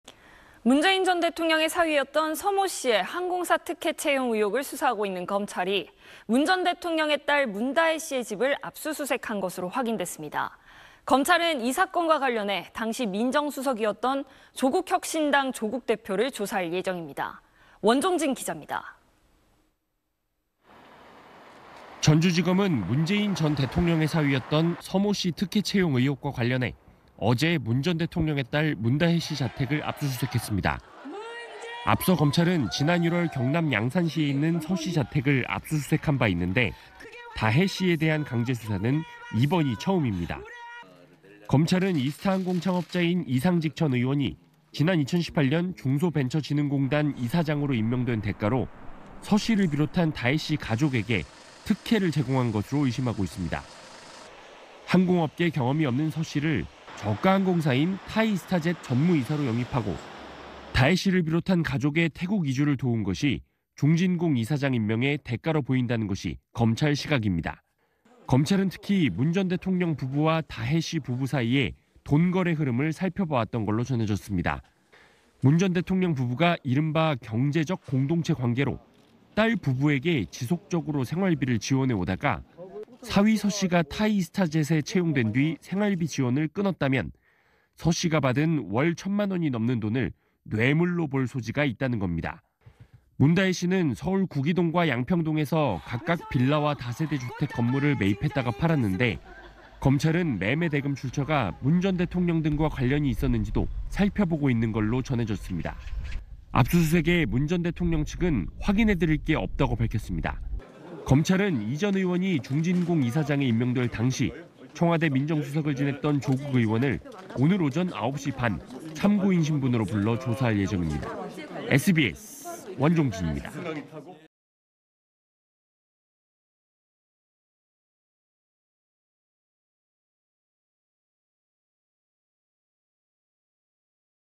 문 전 대통령 딸 자택 압수수색…조국 오늘 조사 / SBS
〈앵커〉

문재인 전 대통령의 사위였던 서 모 씨의 항공사 특혜 채용 의혹을 수사하고 있는 검찰이 문 전 대통령의 딸 문다혜 씨의 집을 압수수색한 것으로 확인됐습니다. 검찰은 이 사건과 관련해 당시 민정수석이었던, 조국혁신당 조국 대표를 조사할 예정입니다.

원종진 기자입니다.

〈기자〉

전주지검은 문재인 전 대통령의 사위였던 서 모 씨 특혜 채용 의혹과 관련해 어제(30일) 문 전 대통령의 딸 문다혜 씨 자택을 압수수색했습니다.

앞서 검찰은 지난 1월 경남 양산시에 있는 서 씨 자택을 압수수색한 바 있는데 다혜 씨에 대한 강제수사는 이번이 처음입니다.

검찰은 이스타항공 창업자인 이상직 전 의원이 지난 2018년 중소벤처진흥공단 이사장으로 임명된 대가로 서 씨를 비롯한 다혜 씨 가족에게 특혜를 제공한 것으로 의심하고 있습니다.

항공업계 경험이 없는 서 씨를 저가 항공사인 타이이스타젯 전무이사로 영입하고, 다혜 씨를 비롯한 가족의 태국 이주를 도운 것이 중진공 이사장 임명의 대가로 보인다는 것이 검찰 시각입니다.

검찰은 특히 문 전 대통령 부부와 다혜 씨 부부 사이의 돈거래 흐름을 살펴봐 왔던 걸로 전해졌습니다.

문 전 대통령 부부가 이른바 '경제적 공동체' 관계로 딸 부부에게 지속적으로 생활비를 지원해 오다가 사위 서 씨가 타이이스타젯에 채용된 뒤 생활비 지원을 끊었다면 서 씨가 받은 월 1천만 원이 넘는 돈을 뇌물로 볼 소지가 있다는 겁니다.

문다혜 씨는 서울 구기동과 양평동에서 각각 빌라와 다세대 주택 건물을 매입했다가 팔았는데, 검찰은 매매 대금 출처가 문 전 대통령 등과 관련이 있었는지도 살펴보고 있는 걸로 전해졌습니다.

압수수색에 문 전 대통령 측은 "확인해 드릴 게 없다"고 밝혔습니다.

검찰은 이 전 의원이 중진공 이사장에 임명될 당시 청와대 민정수석을 지냈던 조국 의원을 오늘 오전 9시 반 참고인 신분으로 불러 조사할 예정입니다.

(영상취재 : 김승태, 영상편집 : 우기정)